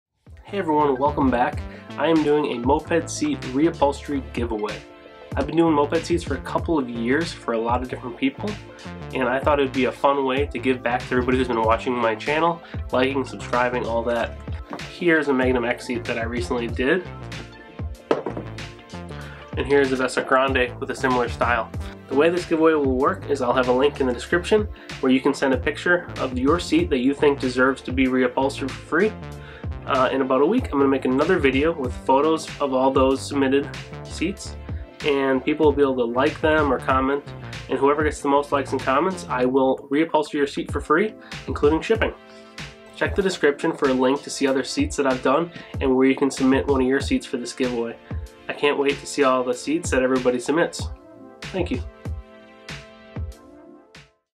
Moped Seat Reupholstery Giveaway
Thank you all who watched and entered!

02/12/2021
I am offering to reupholster your moped seat for free.
Next week I will be making a video with the submissions and you can comment on which seat you think deserves it the most.

After a winner is chosen I will reupholster that seat for free.